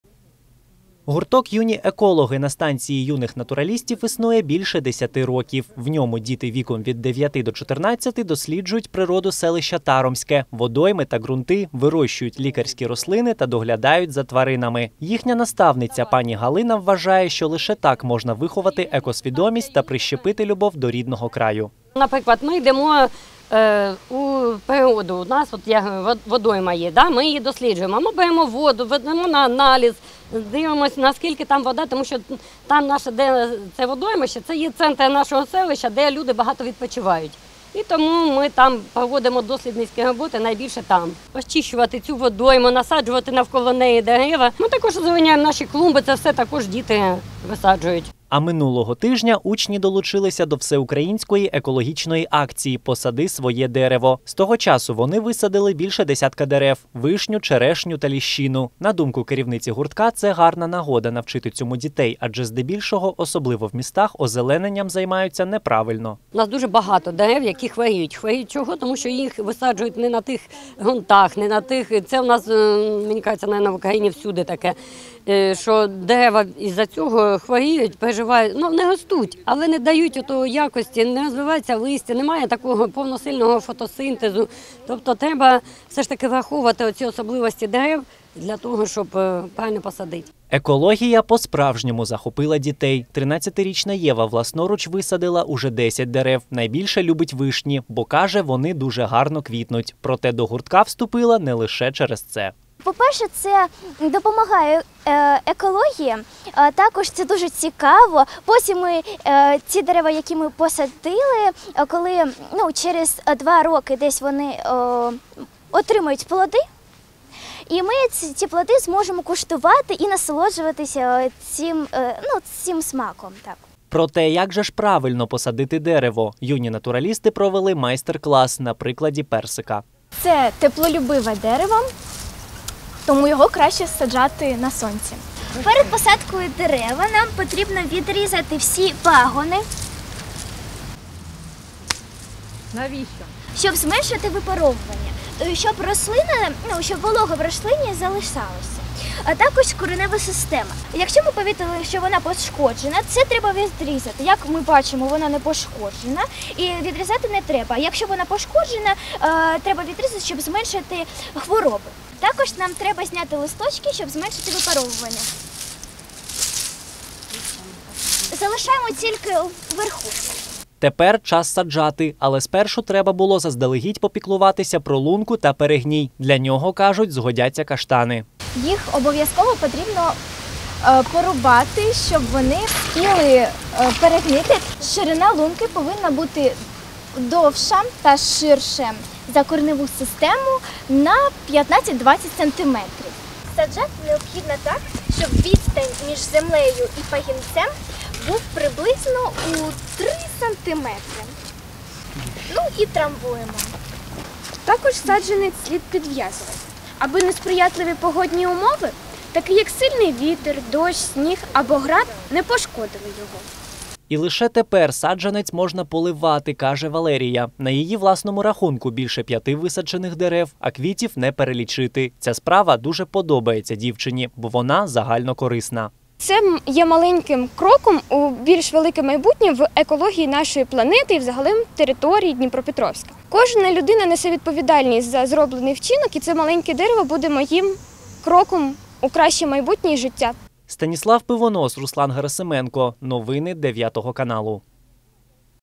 Секрети озеленення: вихованці гуртка «Юні екологи» показали як висаджувати плодові дерева
Вихованці дніпровського гуртка «Юні екологи» долучилися до всеукраїнської акції «Посади своє дерево». Офіційно ініціатива розпочнеться 17 жовтня, але  діти вирішили не гаяти часу і вже почали озеленювати селище Таромське – саме там і розташована їхня станція. Але ця справа – не така проста, як здається, і має цілу низку тонкощів.